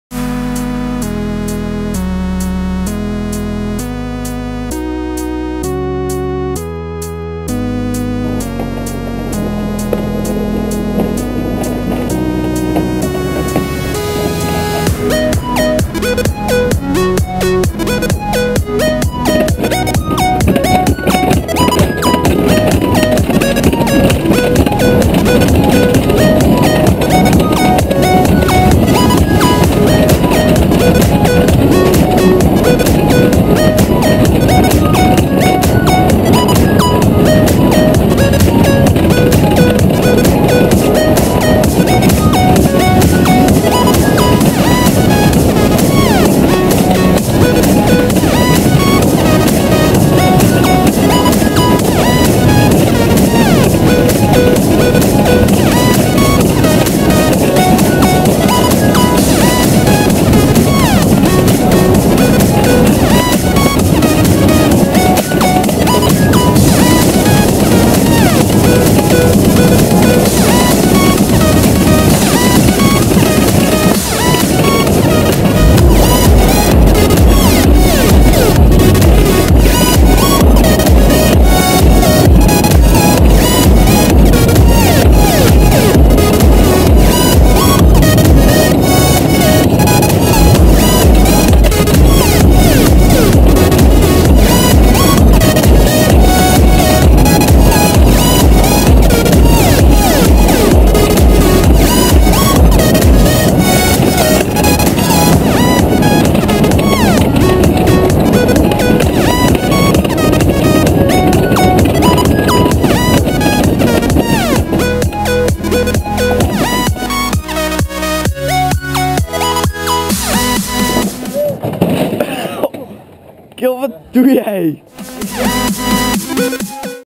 Summer Bobsleigh Course + Funny fail (Caught with GoPro in Germany)
As a second vacation I went to Germany with some friends.

Somehow, YouTube Really knows how to f•ck up the quality of a video, enjoy anyway ;)
(Video rendered in 27mbps).

Music info:
Follow Ahrix: